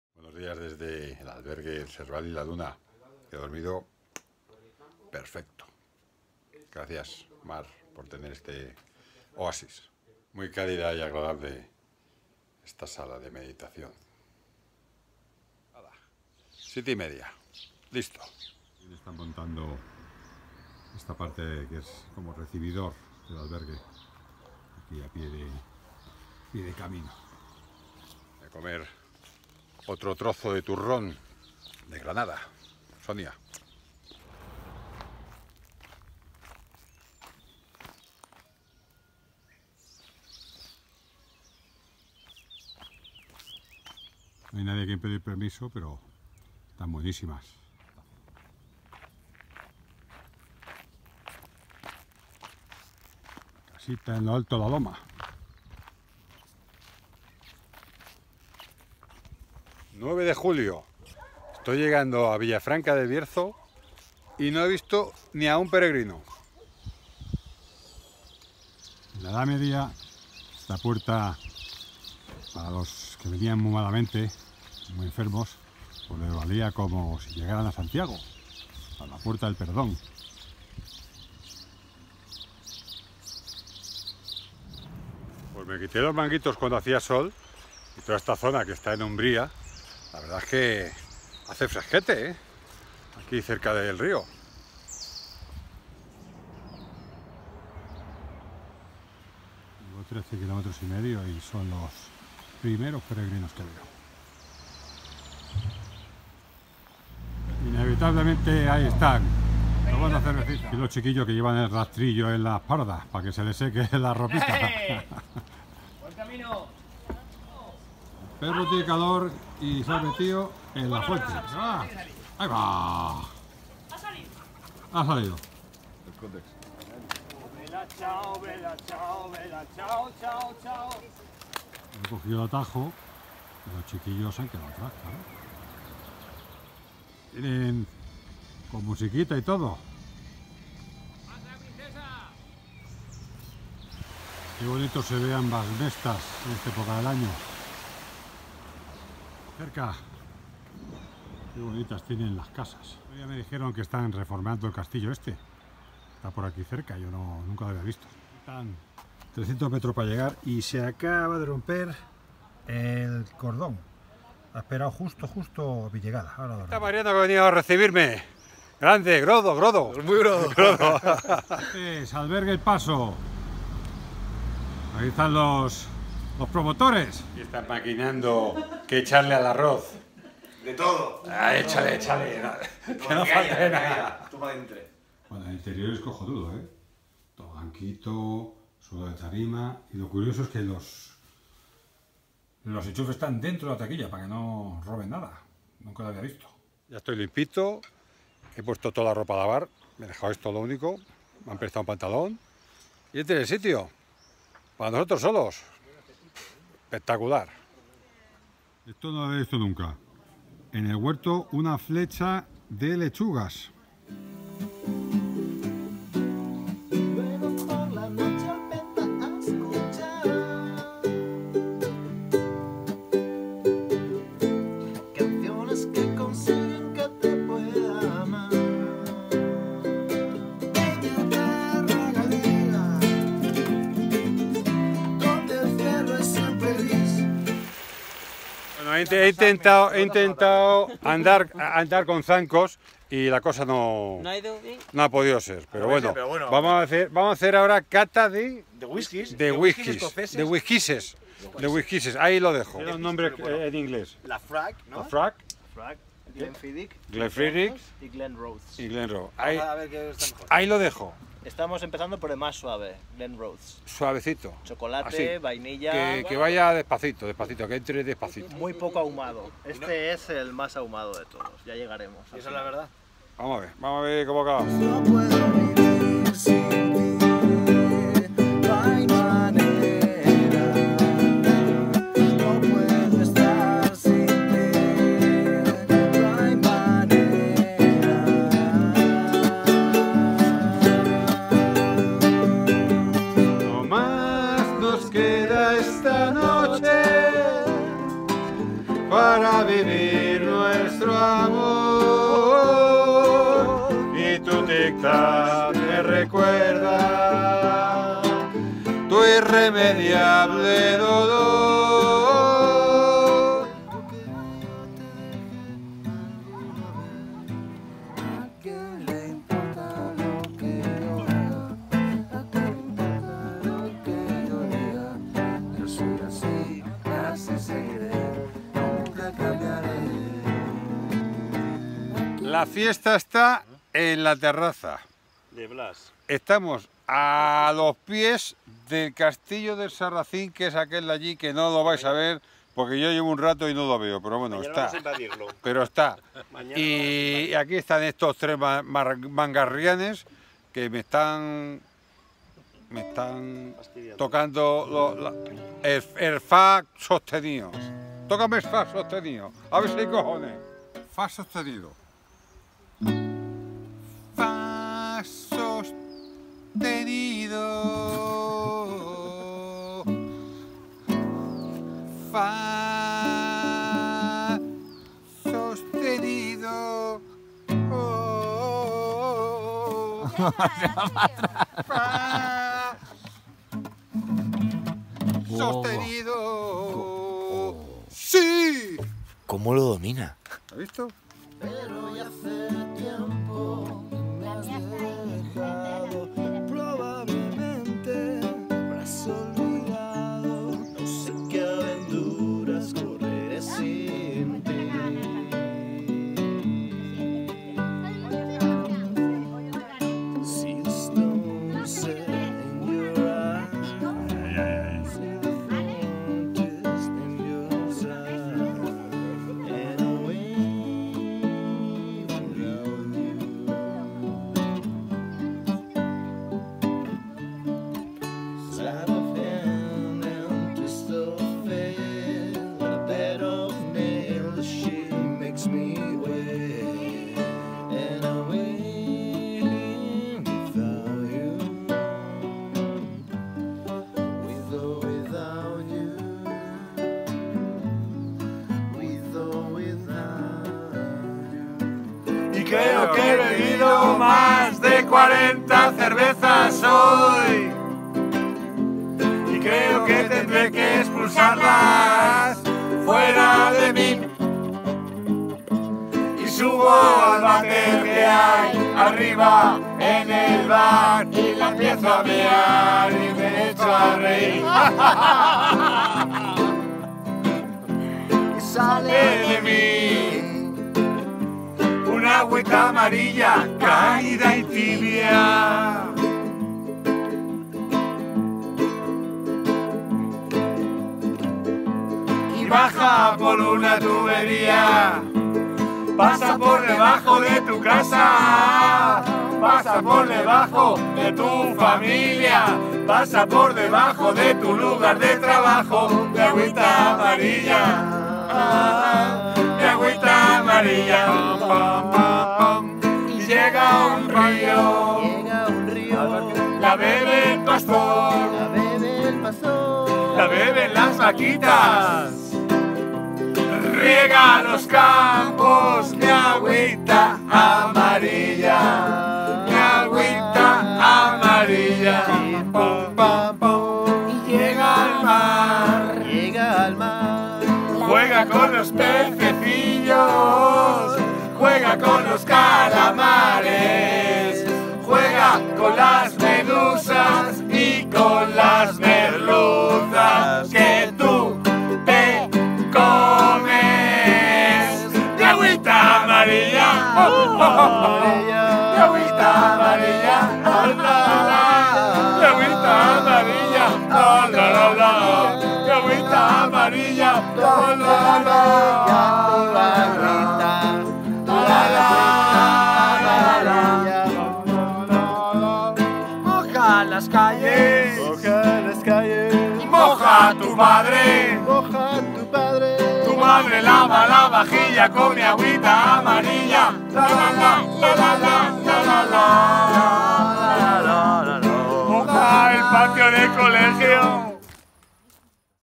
Camino 33º Francés Julio 2018 Parte 6ª
De Pieros a Vega de Valcarce el 9 de Julio de 2018...